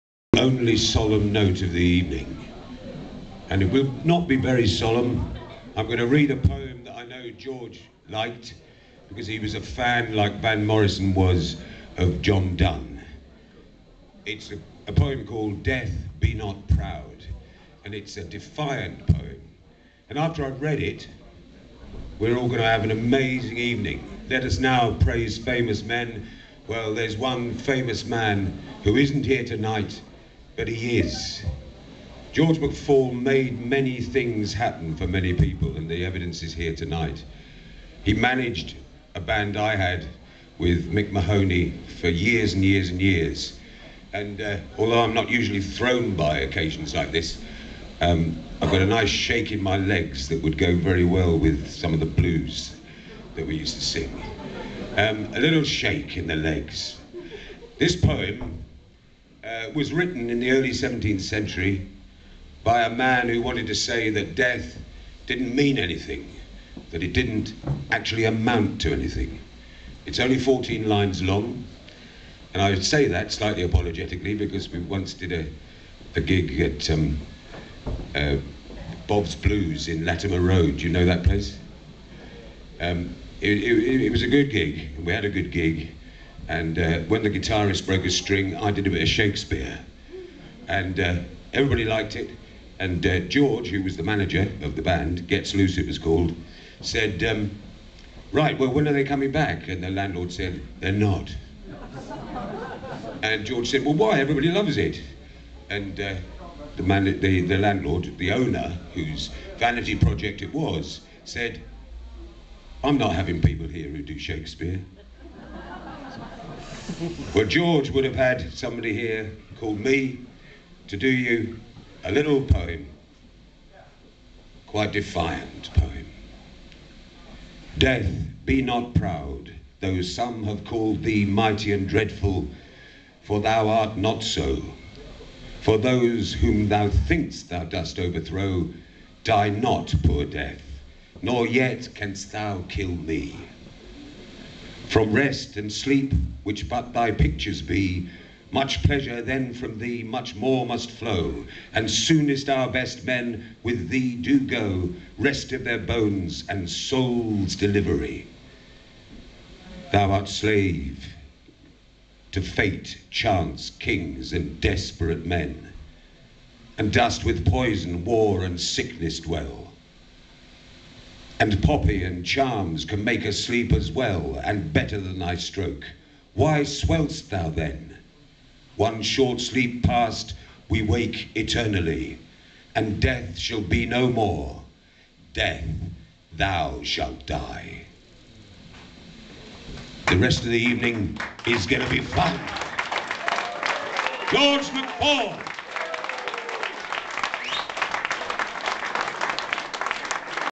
Rob Brown - Memorial show for George McFall at The Boom Boom Club, Sutton, Surrey. UK. 31.10.2010
Memorial show for George McFall at The Boom Boom Club, Sutton, Surrey. UK. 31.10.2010.
George McFall 19th March 1949 - 7th October 2010.May he Rest in Peace

All funds raised, going to George's wife Sheila, and the cancer charity of her choice.